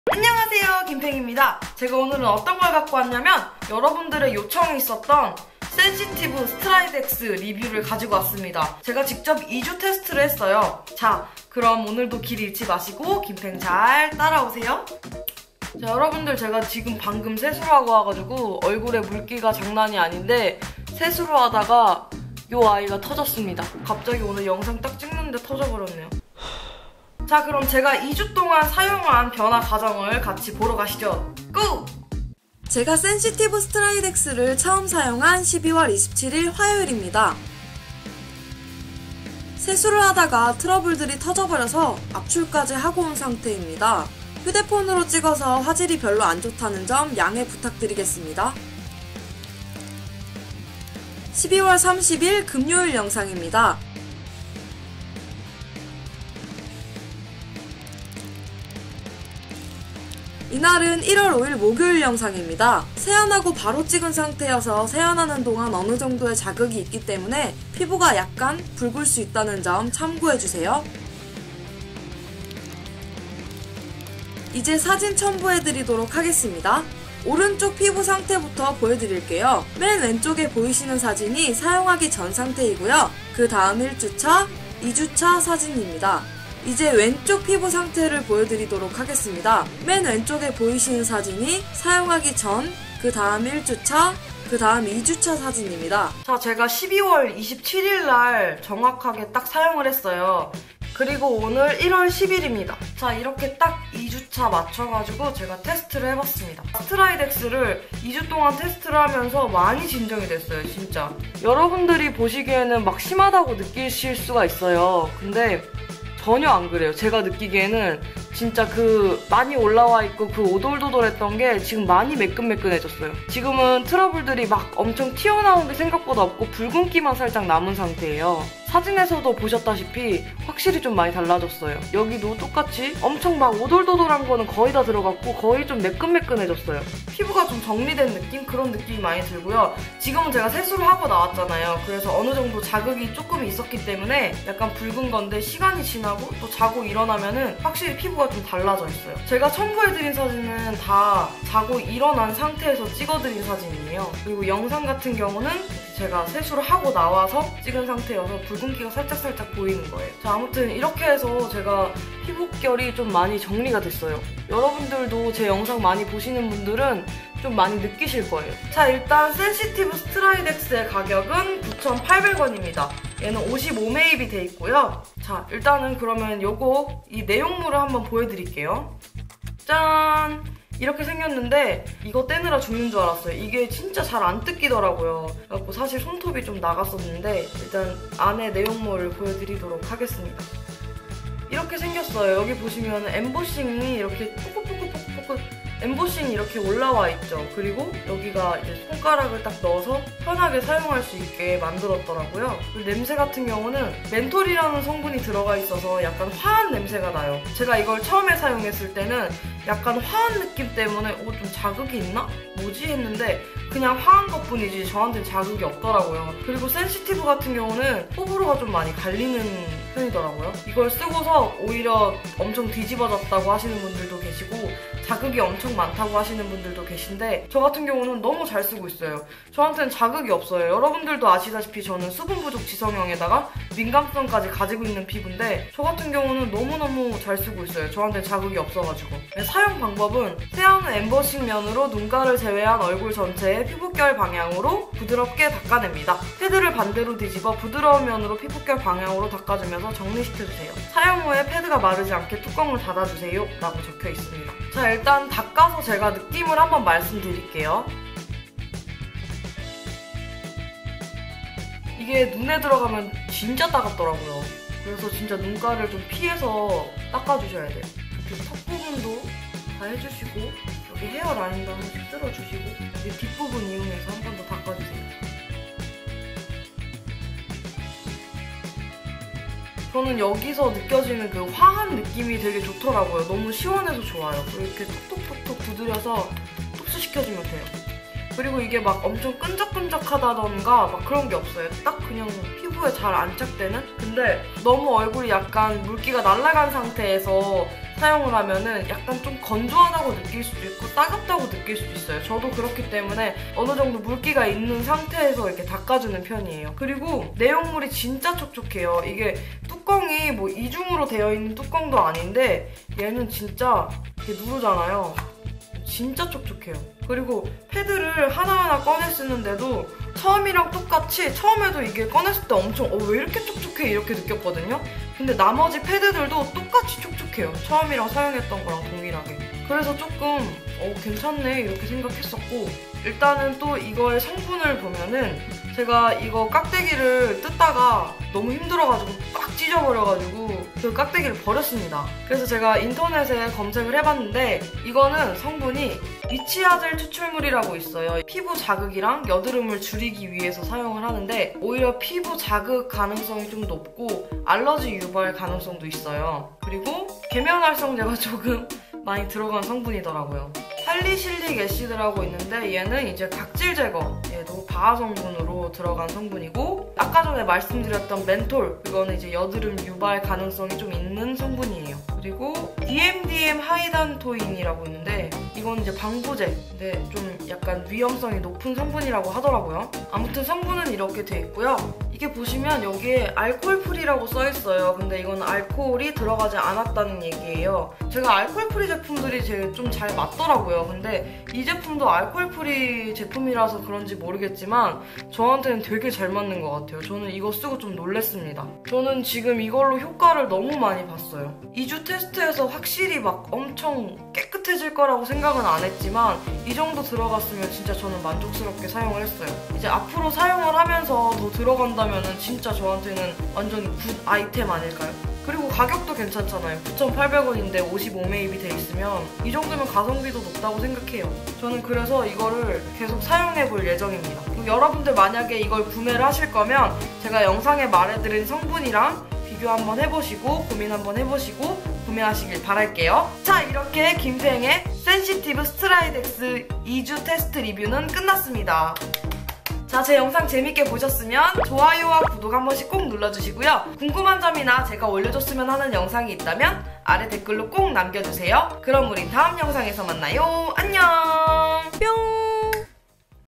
스트라이덱스 센시티브 패드 2주 테스트 솔직 리뷰 l 2주동안 피부가 달라졌을까요?? 스트라이덱스의 성분은 어떤게 들어있을까요? 리얼 후기& 피부변화 궁금하면 들어와!- [귄펭]
안녕하세요 뷰티 크리에이터 귄펭입니다 !

귄펭의 다양한 영상이 궁금하다면?

올리브영 센시티브 스트라이덱스 9,800